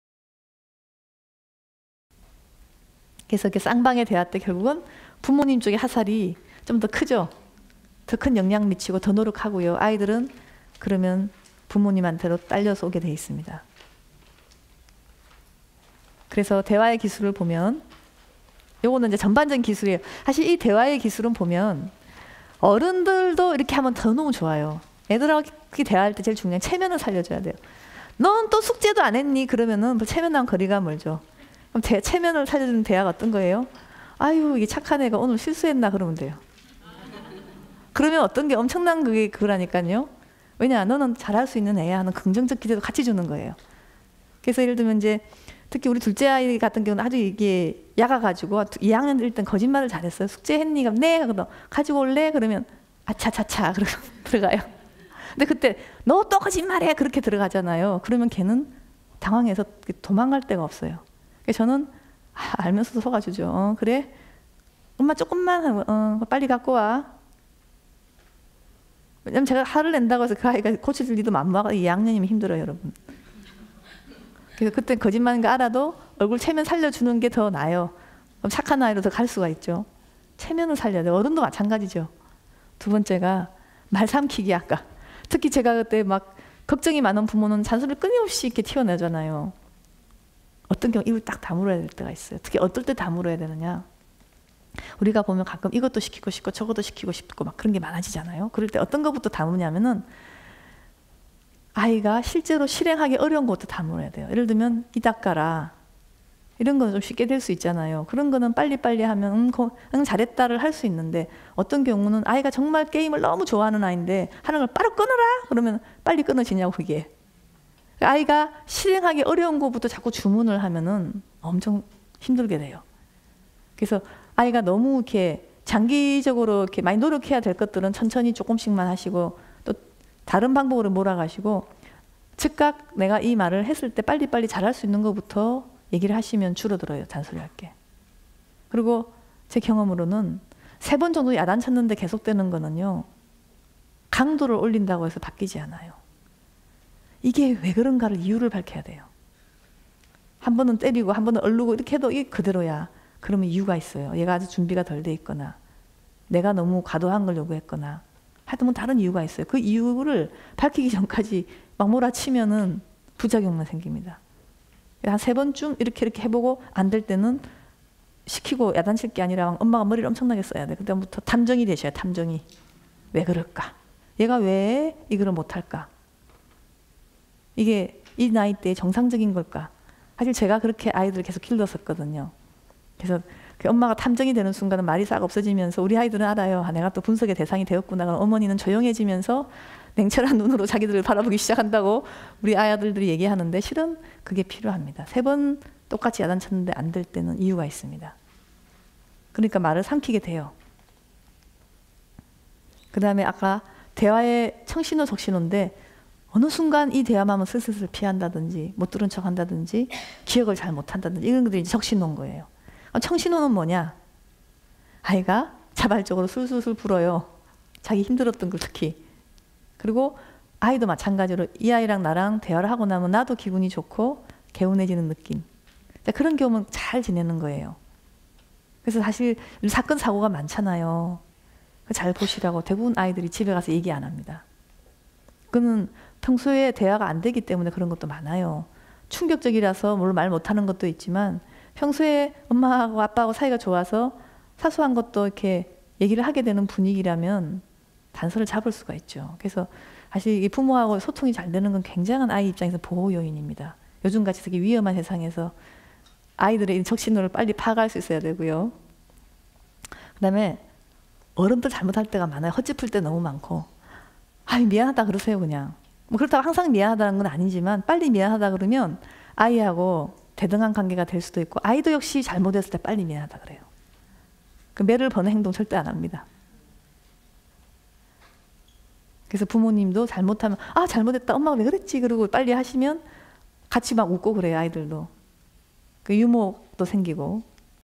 현명한 부모를 위한 대화법2- 우리 아이와 함께하는 감성 커뮤니케이션_신의진 연세의대 정신건강 의학과 교수_9 자녀와의 대화의 기술 I
자녀와의 대화 어렵기만 할까?
아이의 기질에 따라 접근과 이해를 달리해야 하기에 부모는 자녀를 잘 파악하기 위한 탐정이 되어야 한다.
자녀와의 대화를 잘 풀어낼 수 있는 구체적인 팁을 들어보고,
발달단계에 맞는 대화법을 알아본다.